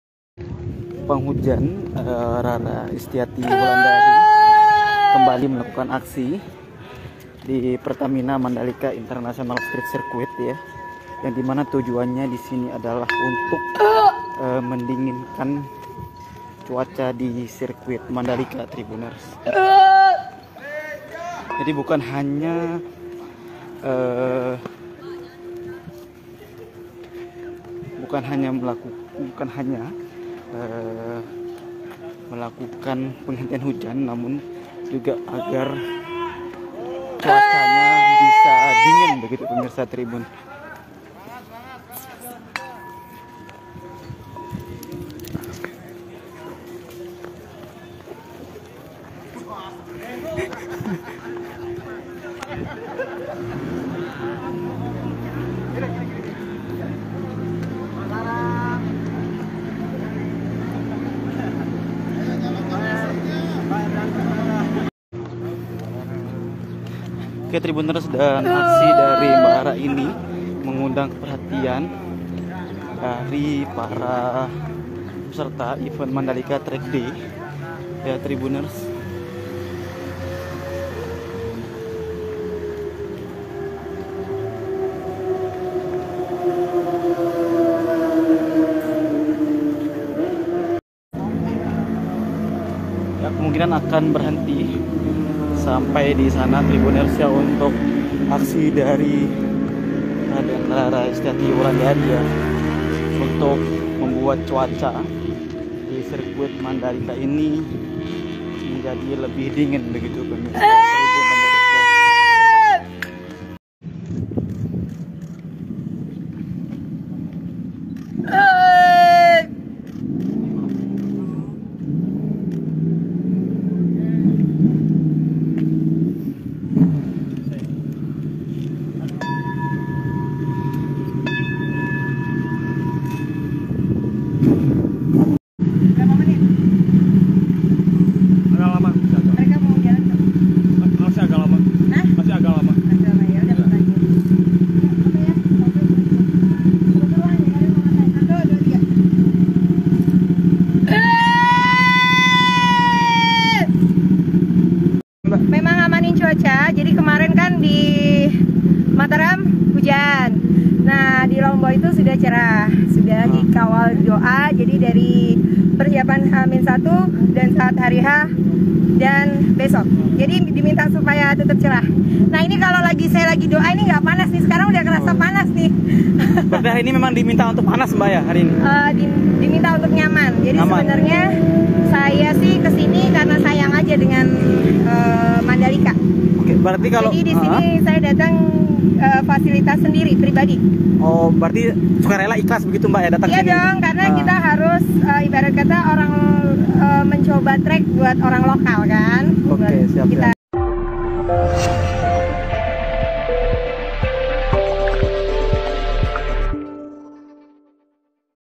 Rara si Pawang Hujan Kembali Beraksi di Sirkuit Mandalika Jelang Balapan
Pawang hujan Raden Rara kembali beraksi berteriak memukul singing bowlnya.

Simak wawancara bersama Rara usai melakukan ritual. Ungkap soal kerjasama dengan MGPA hingga pergeseran awan di sirkuit Mandalika.(cos)